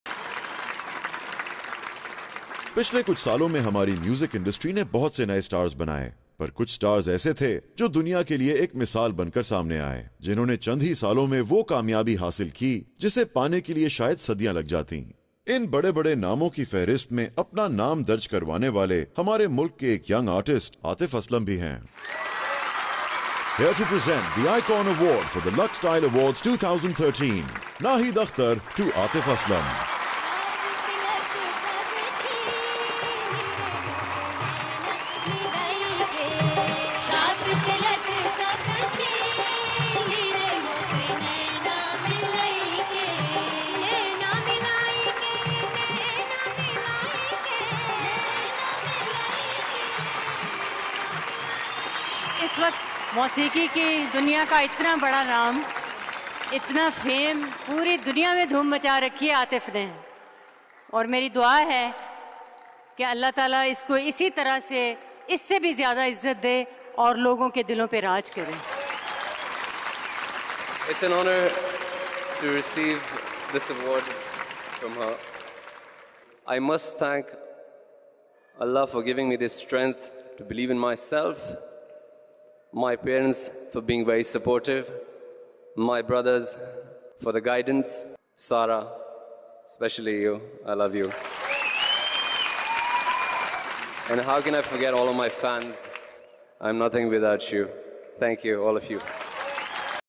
Atif Aslam receving The Music Icon Award in LSA 2013
Atif expresses his love for his wife Sara and thanked his fans after receiving The Music Icon Award from Naheed Akhtar in Lux Style Award 2013